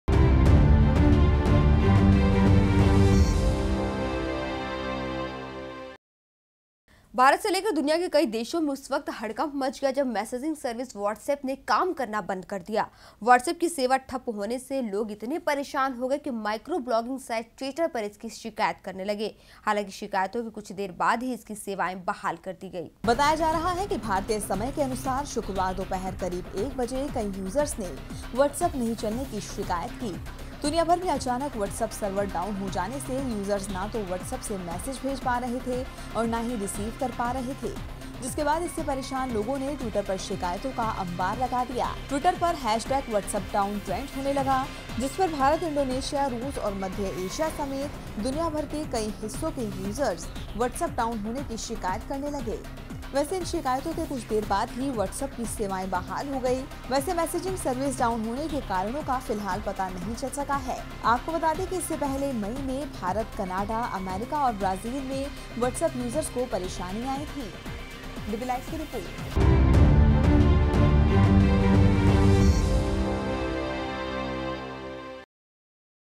Whatsapp हो गया बंद ! |Whatsapp Services Reportedly Non-functional In Many Parts Of India
भारत से लेकर दुनिया के कई देशों में उस वक्त हड़कंप मच गया, जब मैसेजिंग सर्विस व्हाट्सएप ने काम करना बंद कर दिया है। व्हाट्सएप की सेवा ठप्प होने से लोग इतने परेशान हो गए है कि माइक्रो ब्लॉगिंग साइट ट्विटर पर इसकी शिकायत करने लगे, हालांकि शिकायतों के कुछ देर बाद ही इसकी सेवाएं बहाल कर दी गई।
बताया जा रहा है कि भारतीय समय के अनुसार शुक्रवार दोपहर करीब 1 बजे कई यूजर्स ने व्हाट्सएप नहीं चलने की शिकायत की। दुनियाभर में अचानक व्हाट्सएप सर्वर डाउन हो जाने से ना तो यूजर्स व्हाट्सएप से मैसेज भेज पा रहे थे और ना ही रिसीव कर पा रहे थे। जिसके बाद इससे परेशान लोगों ने ट्विटर पर शिकायतों का अंबार लगा दिया। जिसके बाद ट्वीटर पर whatsappdown ट्रेंड होने लगा, जिस पर भारत, इंडोनेशिया, रूस और मध्य एशिया समेत दुनिया भर के कई हिस्सों के यूज़र्स ने व्हाट्सऐप डाउन होने की शिकायत करने लगे। वैसे इन शिकायतों के कुछ देर बाद ही व्हाट्स एप्प की सेवाएं बहाल हो गई। वैस मैसेजिंग सर्विस डाउन होने के कारणों का अभी तक पता नहीं चल सका है। आपको बतादें कि इससे पहले मई में भारत, कनाडा, अमेरिका और ब्राजील में व्हाट्सएप यूजर्स को परेशानी आई थी।